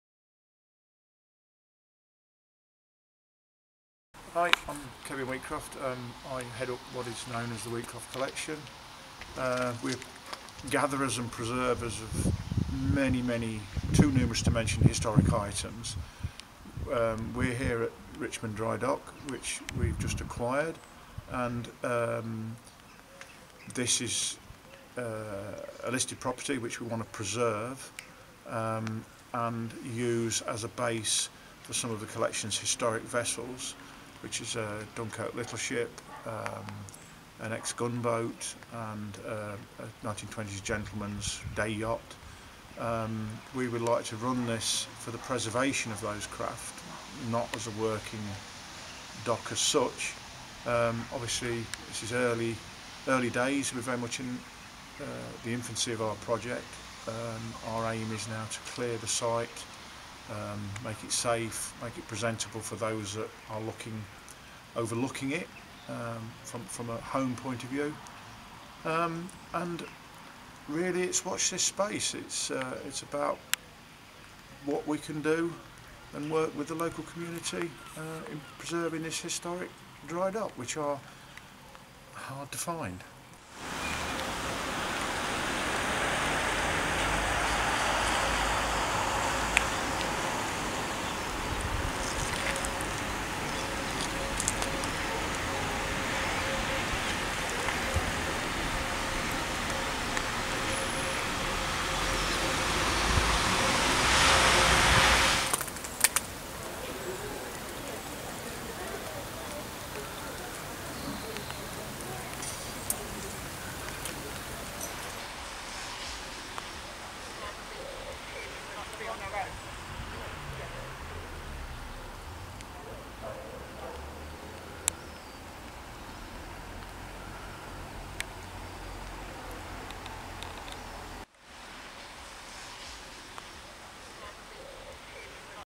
Military collector Kevin Wheatcroft buys Appledore’s Historic Richmond dry dock for £1.2M
Kevin Wheatcroft of the Wheatcroft Collection has bought Richmond dry dock in North Devon's Appledore, to store and refurbish his historic vessels.

Appledore’s Richmond dry dock will become home to a wartime German E boat and other historic vessels after it was sold to military collector Kevin Wheatcroft.

The multi-millionaire is a living history expert and owner of the Wheatcroft Collection, one of the world’s largest private collections of Second World War memorabilia that includes many German vehicles plus those from other nations.

Mr Wheatcroft intends to use the dry dock at Appledore to store historic vessels including S-130, the German Schenell-Boote or S boat that served in the war as well as a captured British prize and part of NATO forces until the 1980s.

The dock was sold by commercial property consultant Kevin Underwood, jointly with local agents Morris and Bott, with an original guide price of £1.2million.

Mr Wheatcroft said: “After we discovered Richmond dry dock and initial talks with local authorities and Historic England, we outlined our plans for the dock as a working home for some of The Wheatcroft Collection’s historic boats, so returning the dry dock to its original intended use.”

“Richmond dry dock is a listed property we want to preserve and use as a base. In our collection there is a Dunkirk little ship, an ex-gun boat and a 1920s gentleman’s day yacht.

“We would like to run this for the preservation of those craft, not as a working dock.”

Mr Wheatcroft owns the famous Donington Park estate and the Wheatcroft Collection is well known in military history circles, containing many German vehicles including Panther and Tiger tanks.

Speaking of his plans for the Appledore site, he said: “Obviously, we’re very much in the infancy of our project.

“Our aim now is to clear the site of the detritus left by the previous owners and make it safe and presentable for those whose homes overlook it, so really it’s watch this space.

“It’s about what we can do in working with the local community in preserving this historic dry dock, which I have to say, are very hard to find.”

The S boat S-130 served during the war before being taken into the Royal Navy until it was returned to Germany in 1957, serving as part of NATO forces until in 1991 it became a houseboat. Mr Wheatcroft purchased it in 2009.